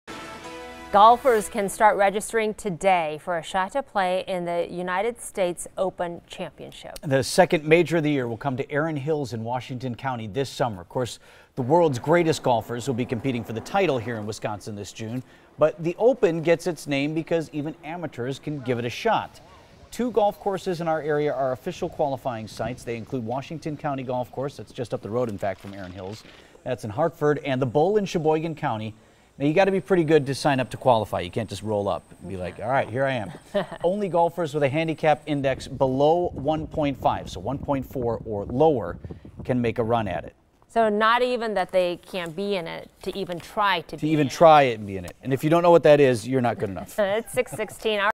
Sign up begins for US Open qualifers
Golfers can begin registering for a shot to play in the United States Open Championship.